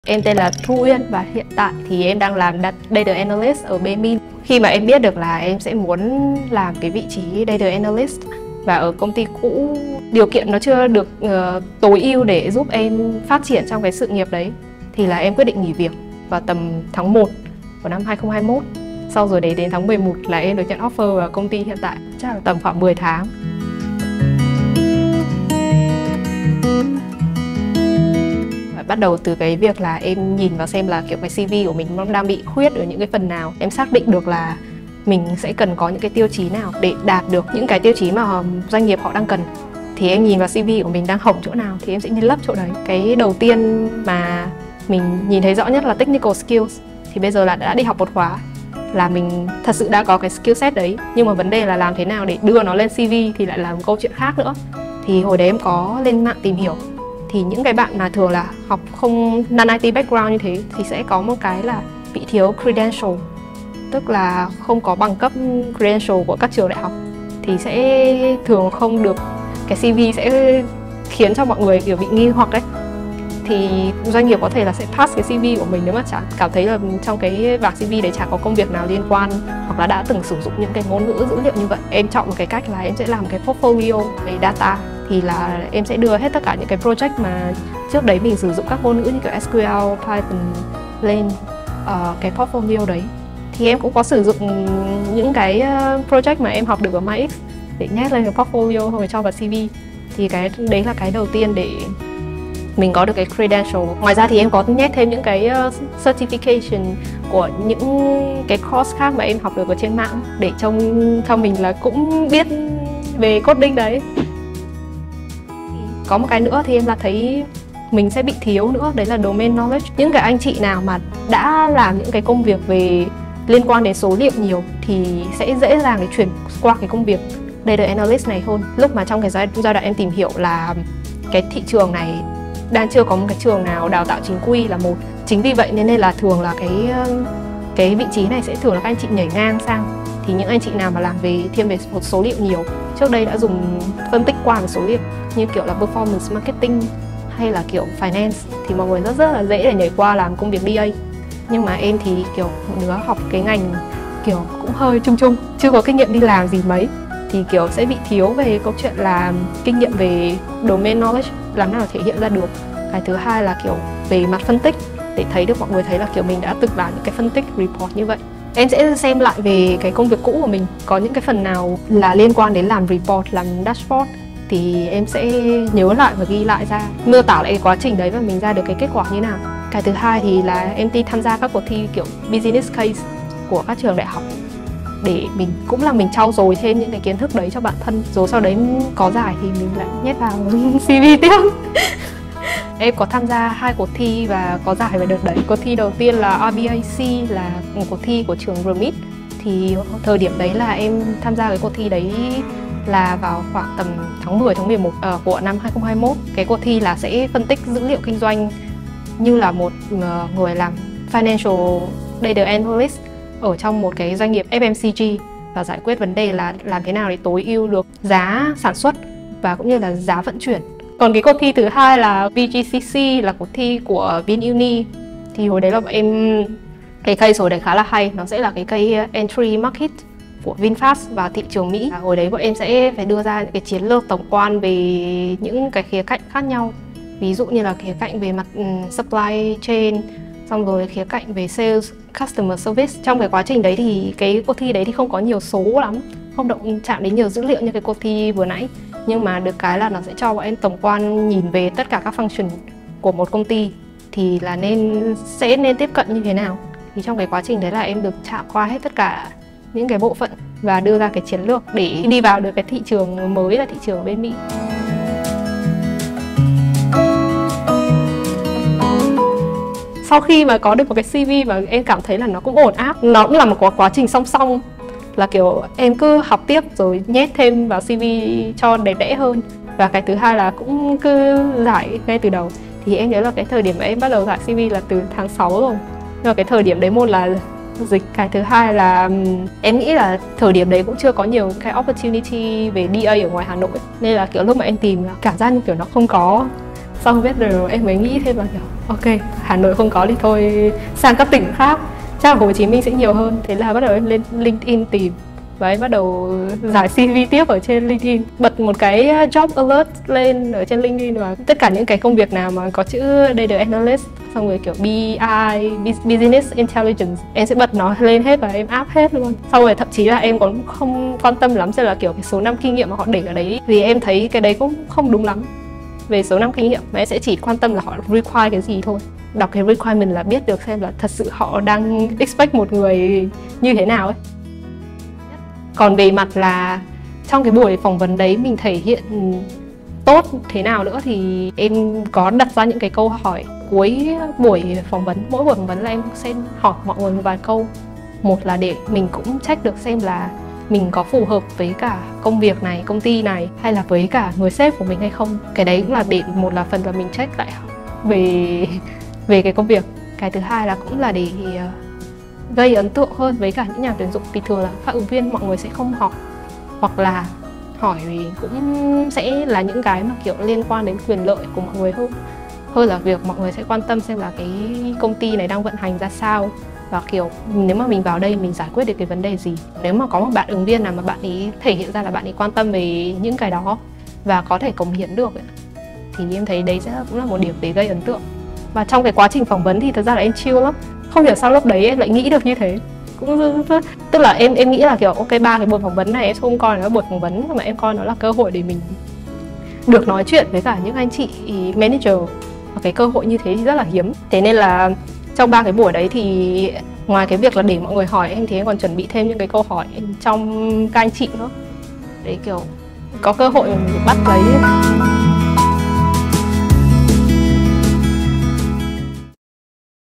Cách làm CV & Ứng tuyển thành công vị trí Data Analyst dù chưa có nhiều kinh nghiệm
Ngành Data Analyst đang rất khát nhân lực, mở ra nhiều cơ hội việc làm cho nhiều người chuyển ngành sang Data dù chưa có nhiều kinh nghiệm.

Tuy nhiên, để dễ dàng tìm kiếm các công việc DA phù hợp, ngoài việc trau dồi kiến thức, bạn cần phải có CV và Portfolio xịn sò để gây ấn tượng với nhà tuyển dụng.

Cùng lắng nghe chi tiết "Cách làm CV & Ứng tuyển thành công vị trí Data Analyst dù chưa có nhiều kinh nghiệm" của bạn Thu Uyên - Data Analyst tại Baemin - Người đã chuyển ngành thành công sang DA chỉ sau 10 tháng.